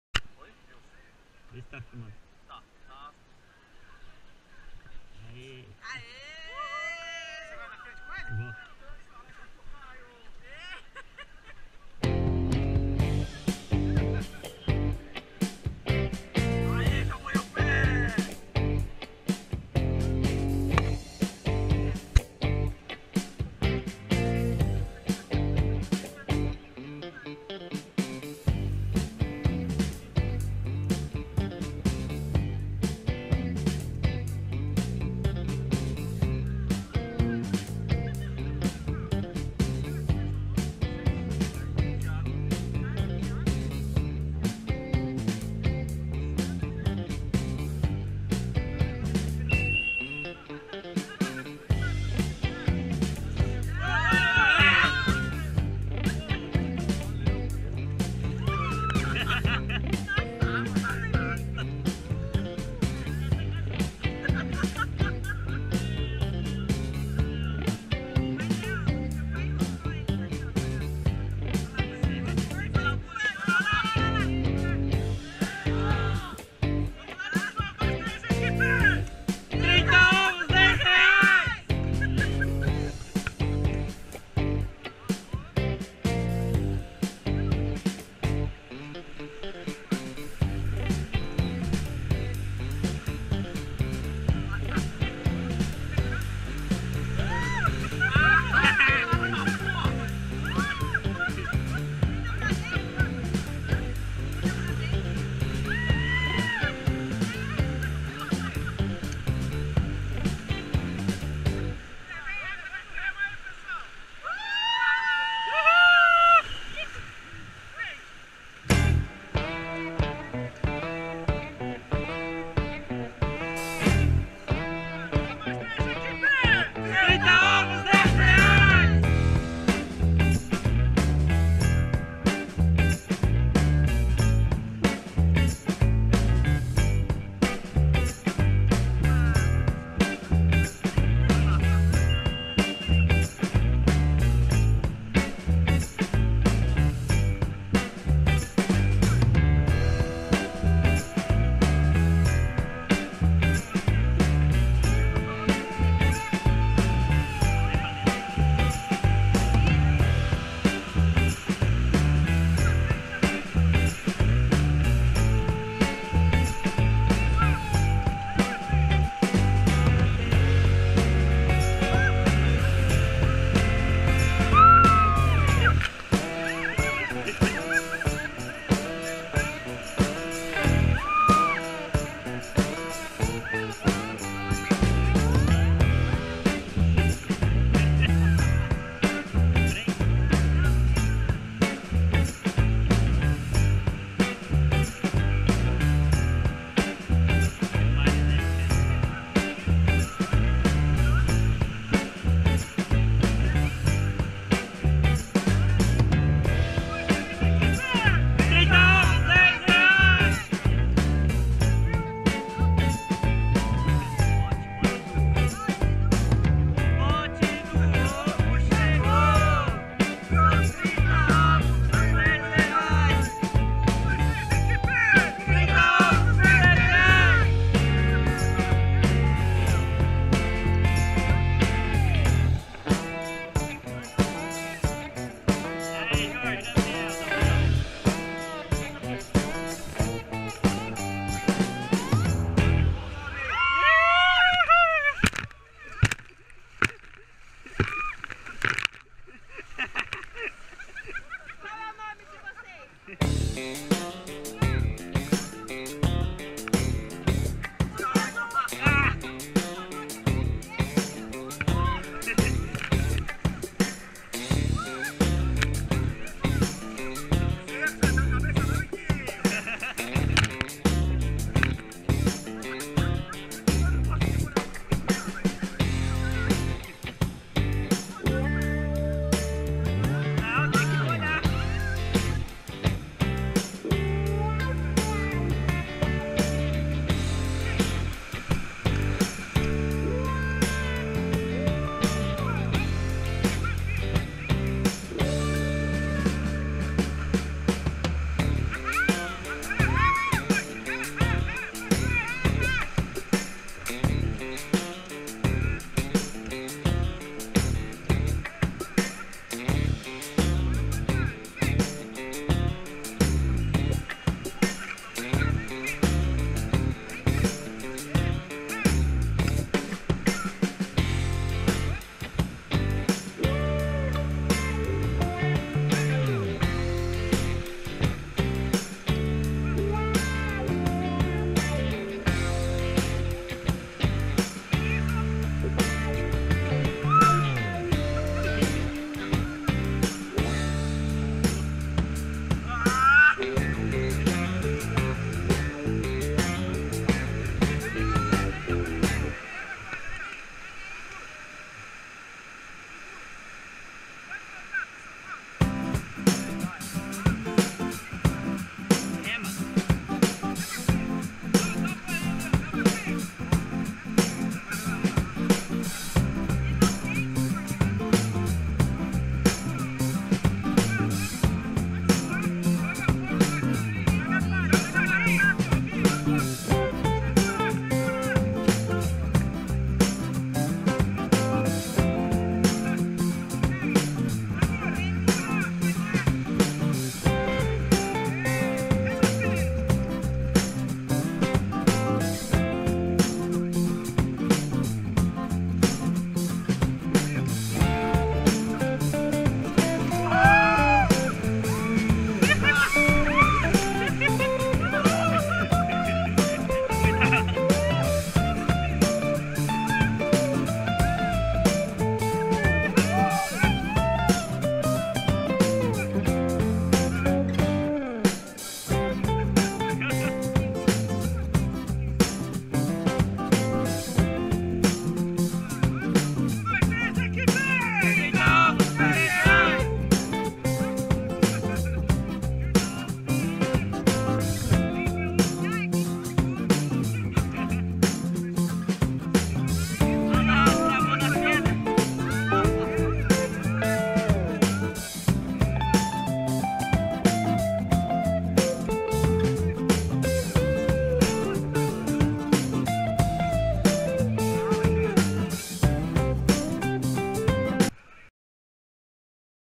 Rafting Socorro-SP (Equipe 30 ovos 10 reais)
rf_spearfishing rafting Rafting realizado com amigos na cidade de Socorro-SP no mês de fevereiro de 2018.